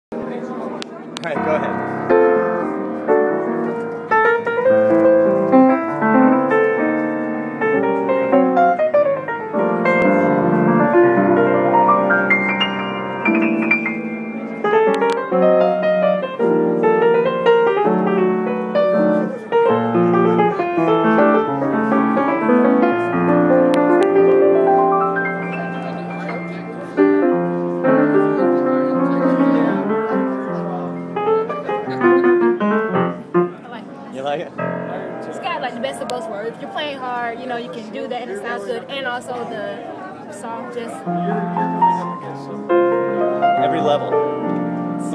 NAMM 2011 Tracy Martin at the Fazioli piano booth (part two)
Keyboard Magazine filmed a group of talented piano players testing out all the top 9 foot grand pianos at the NAMM show. Here we have Tracy Martin checking out one of Fazioli's stellar grand pianos. Watch in HD for the best quality!